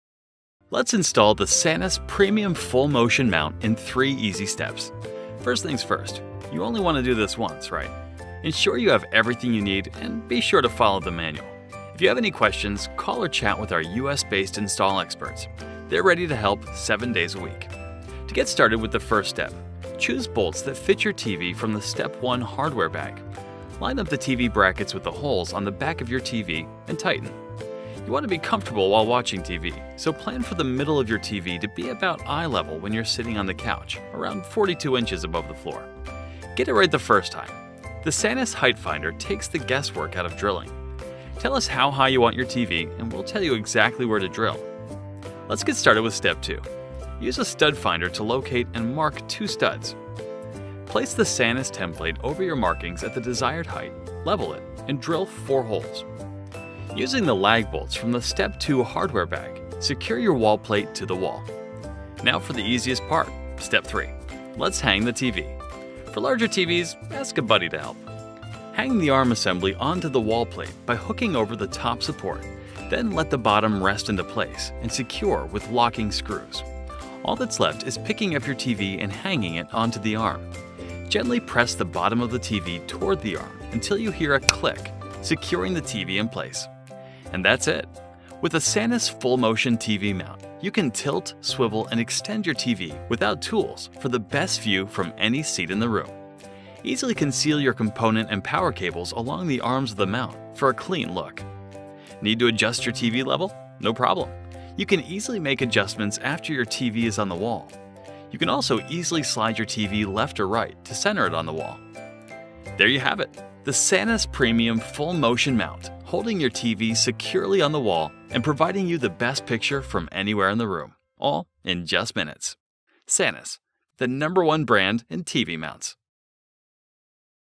SANUS VMF620 Install Video
The VMF620 full-motion+ mount lets you enjoy a great picture no matter where you watch. Tilt, swivel, extend and level your TV without the need for tools. Unsightly cables stay concealed, and tool-free assembly ensures a quick and easy installation. TV can slide from side-to-side for perfect positioning on the wall, and safety tab provides a safe and easy connection you can hear.